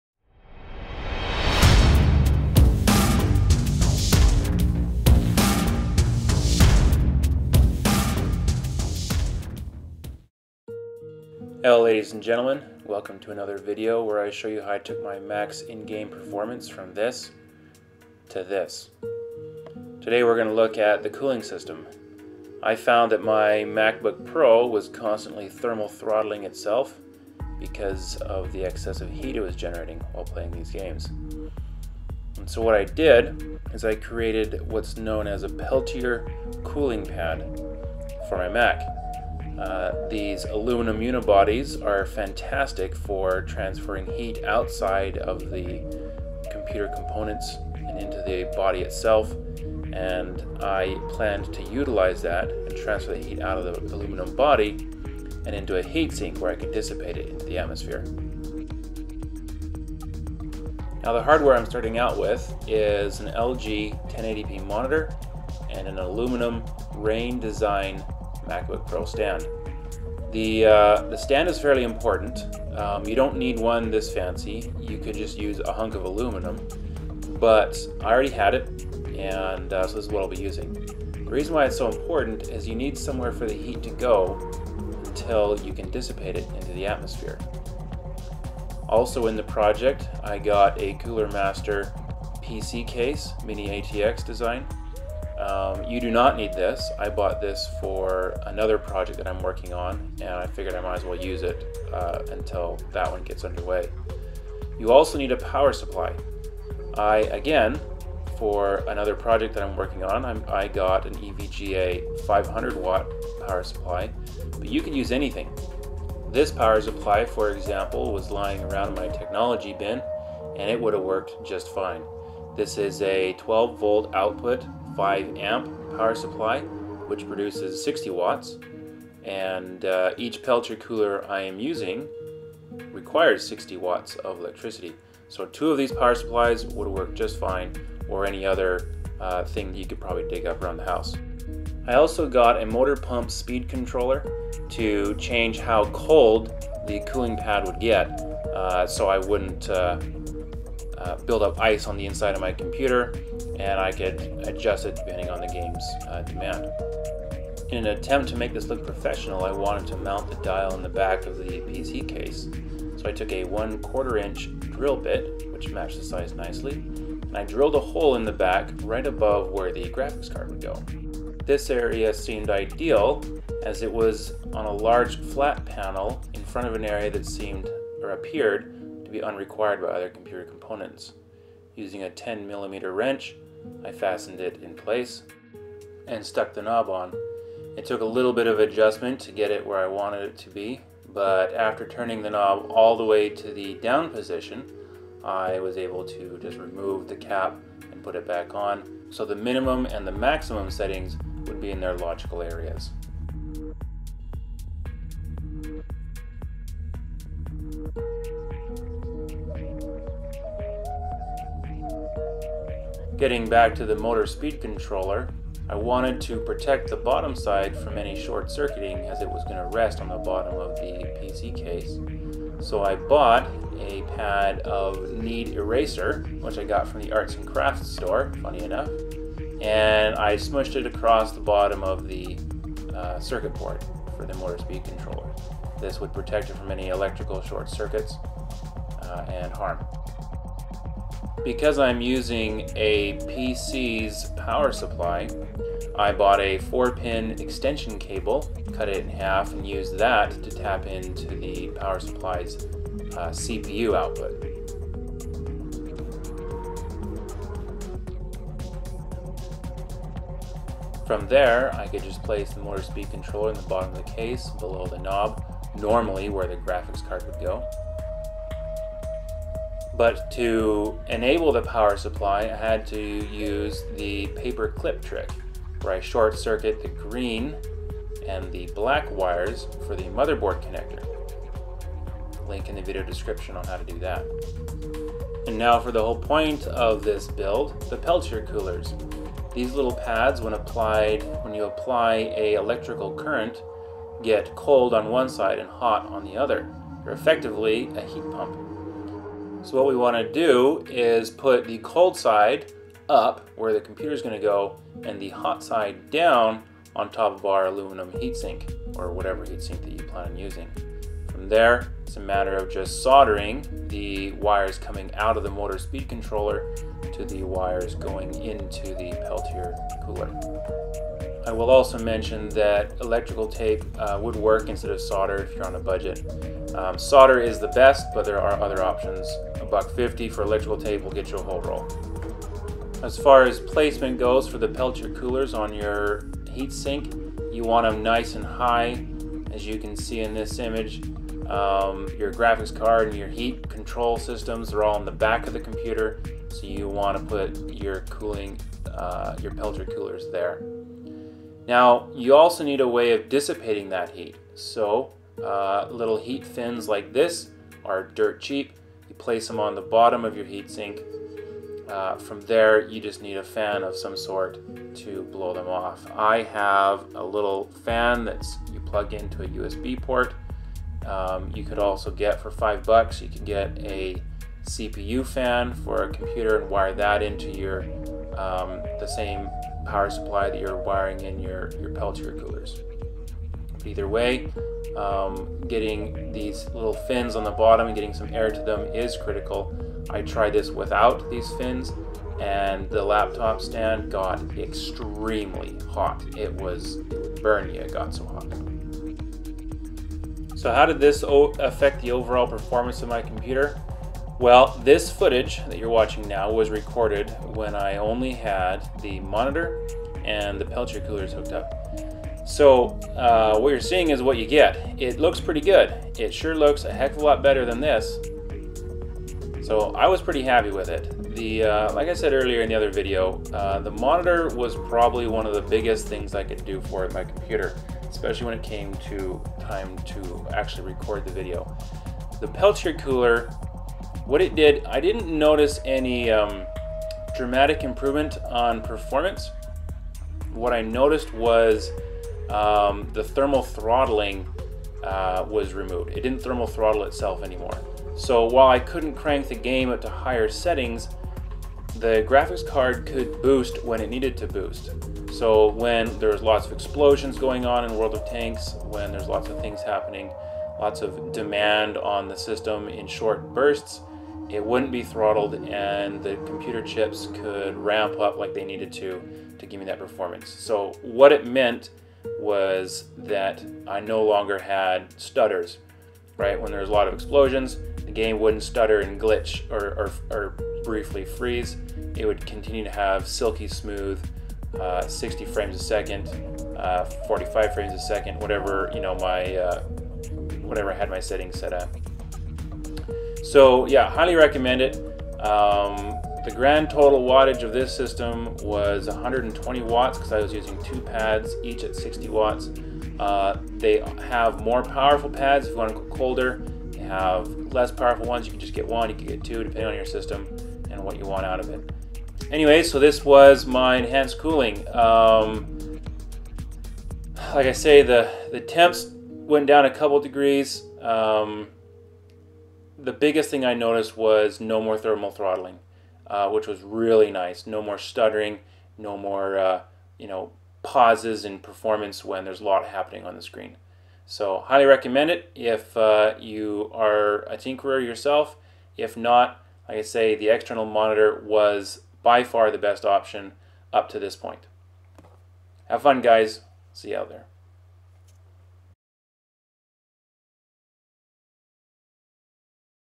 Empowering My Mac - Enhanced Cooling
I refered to another video a couple times, well it hasn't actually been uploaded yet, so no, your not crazy. It basically talks about how playing the game on a 1080p monitor instead of the built in 2880 x 1800 monitor made a HUGE difference. Even if your settings are set lower, the computer still has to power all those pixels. Dropping the raw pixel count by almost 75% made a big difference. So playing on an external monitor with the MacBook Pro in clamshell mode is ideal.

The total REQUIRED cost of this build is $50, mine was a little more because I wanted it to look pretty and have some extra functionality (i.e. the motor speed controller)

(EU Server)